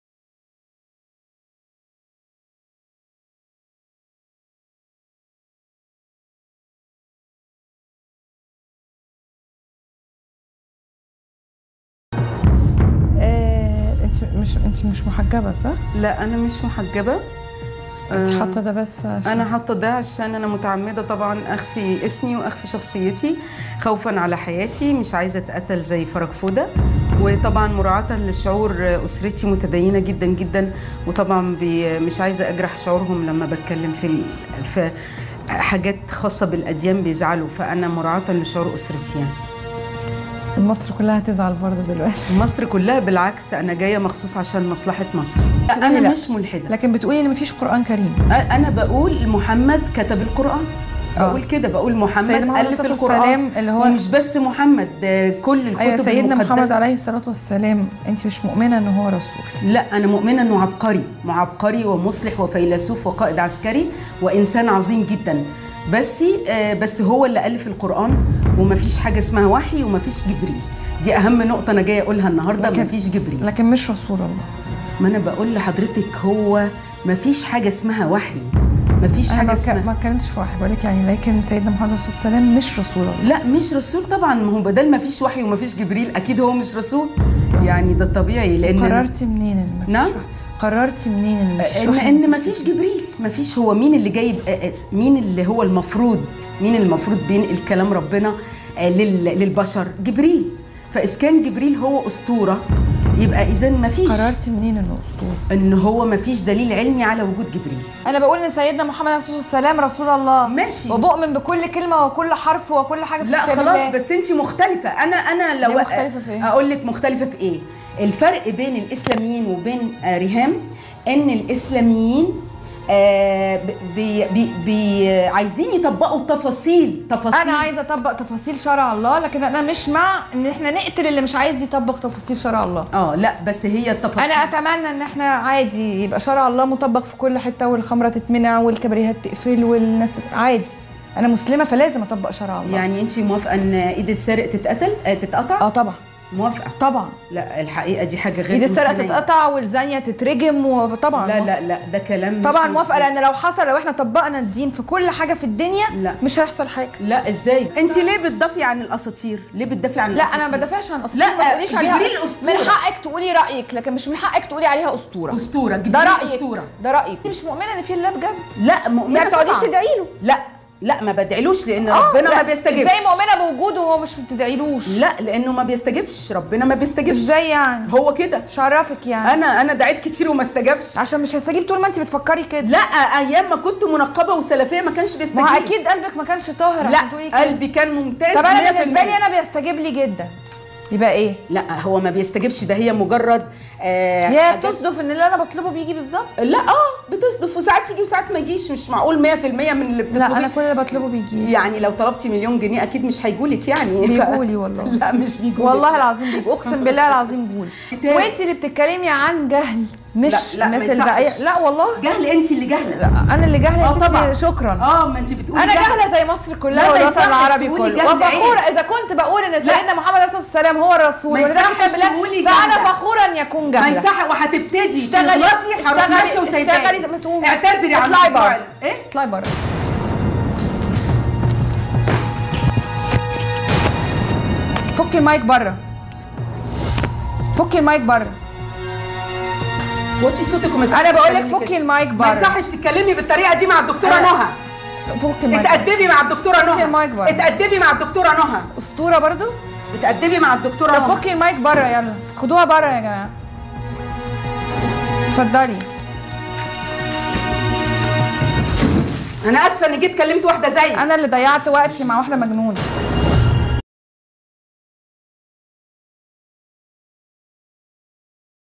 Awantura o Mahometa w egiskiej TV
Egipska lekarka przyznała się w telewizyjnym studio, że nie wierzy w objawienie, a Koran został napisany przez Mahometa.  Obrażona prezenterka wyrzuciła ją ze studia.